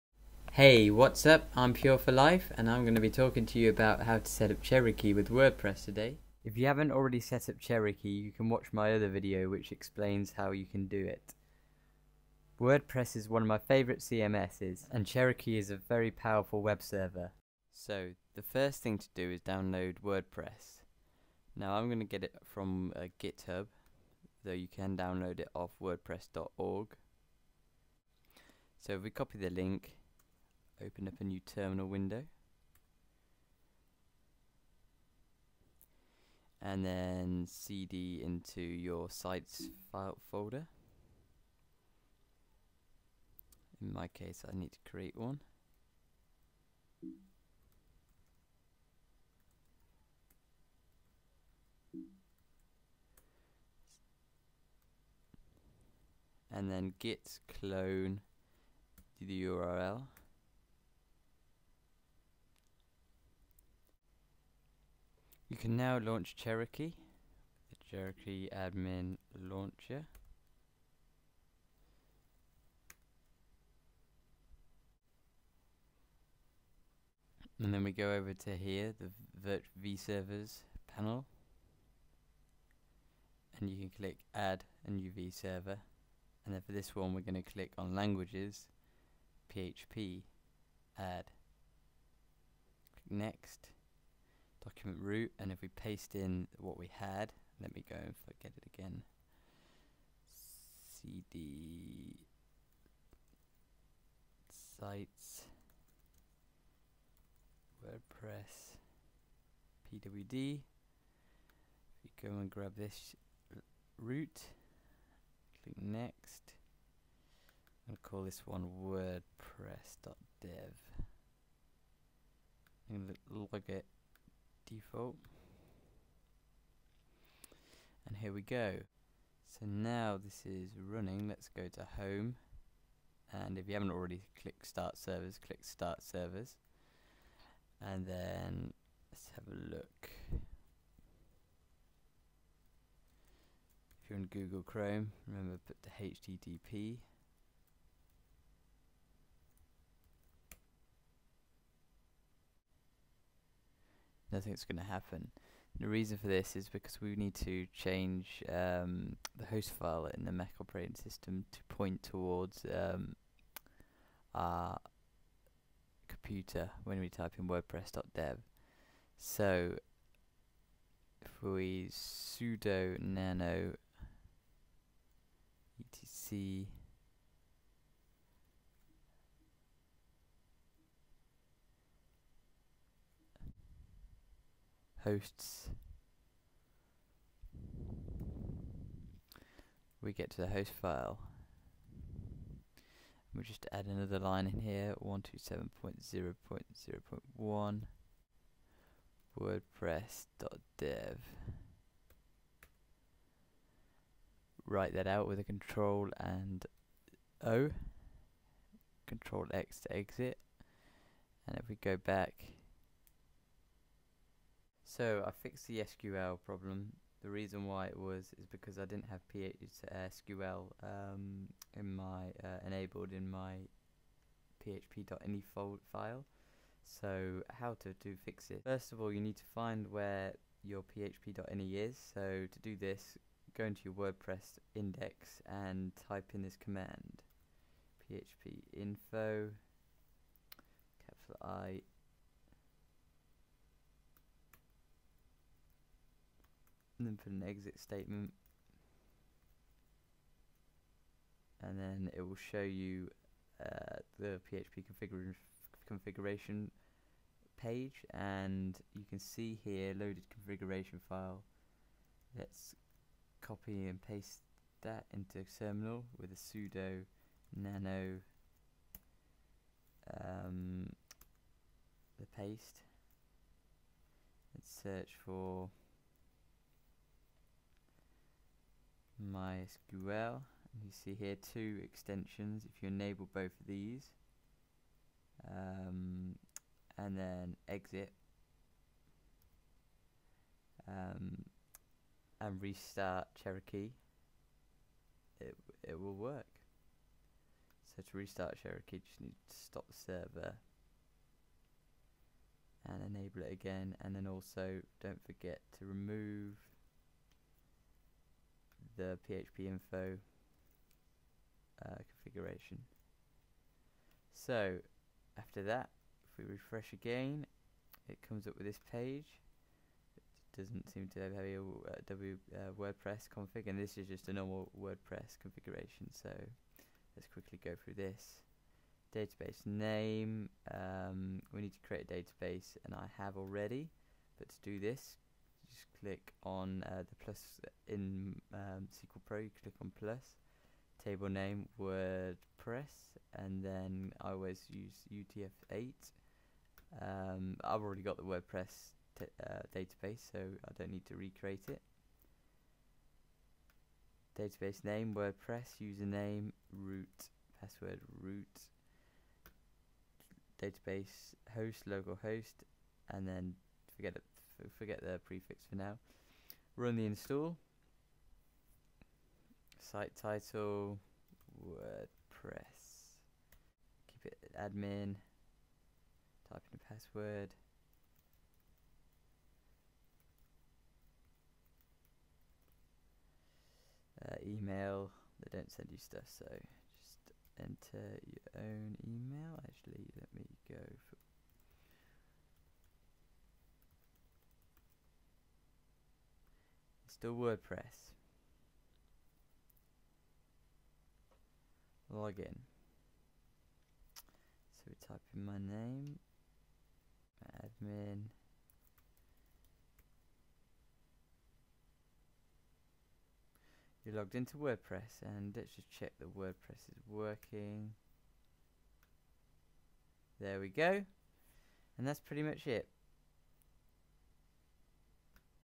Setup Wordpress on Cherokee webserver
How to setup Wordpress on the Cherokee webserver

Step 2 - run cherokee
cherokee-admin-launcher

Step 3 - create new vserver
languages/php
paste the root of your wordpress folder

Step 4 - if its not already running run your cherokee servers

Step 5 - edit host file
sudo nano /etc/hosts
add line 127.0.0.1 'your virtual servers name'

Step 6 - go onto virtual servers name'

Step 6.5 - if you get the mysql not running error
go to your php.ini
enable the mysql lines

Step 7 - create a wordpress database

Step 8 - run through the wordpress configuration